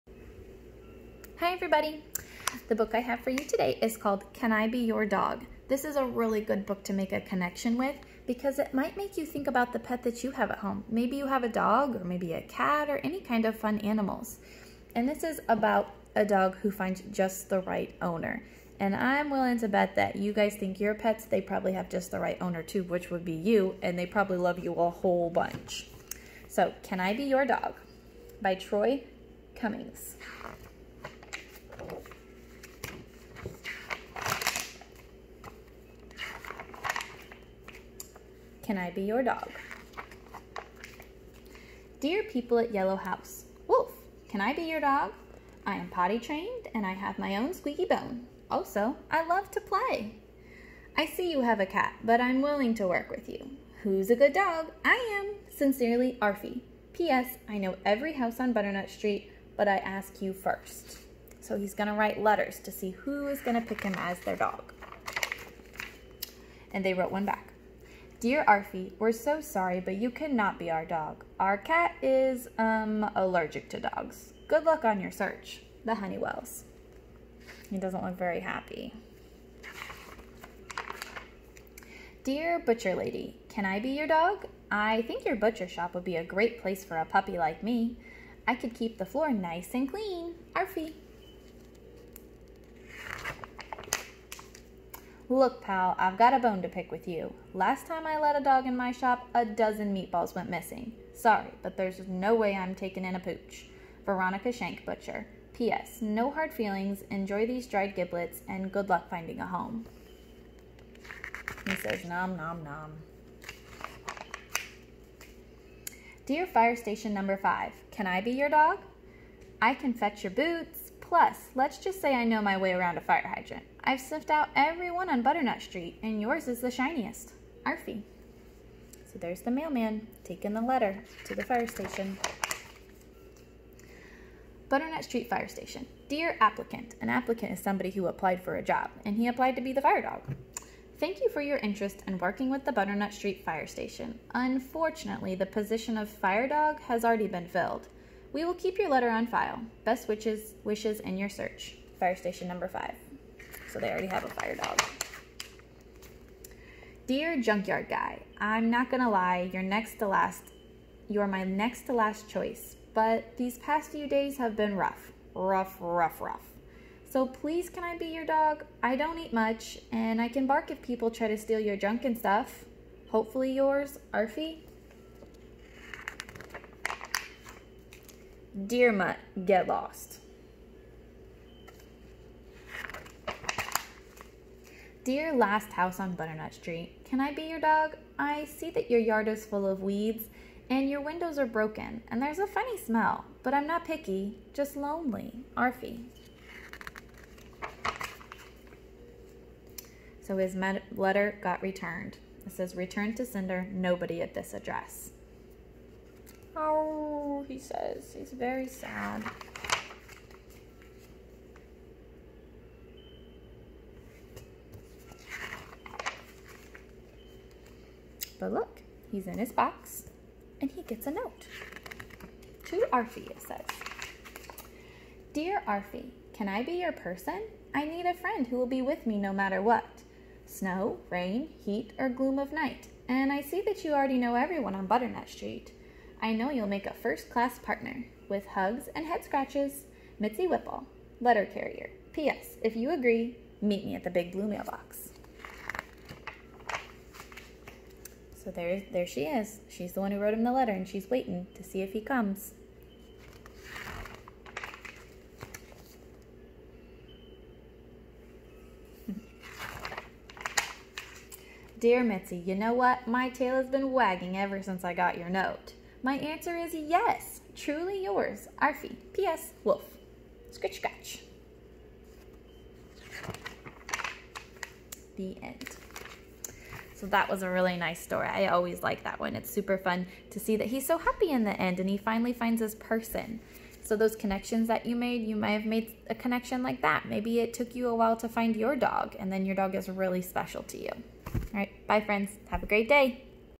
Read Aloud by Mrs. Hamilton- Can I Be Your Dog
Do you have a dog? You might make a connection with this sweet story about a dog finding the perfect person!